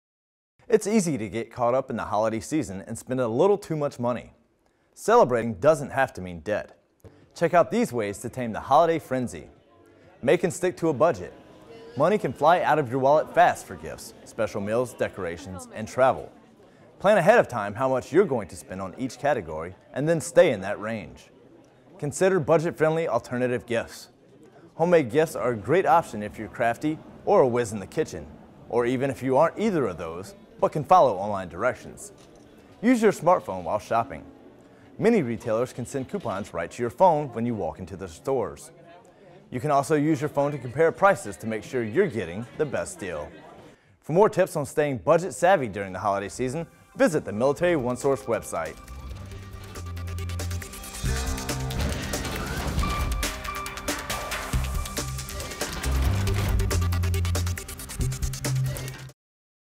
Holiday Spending
How to budget your money during the holiday season.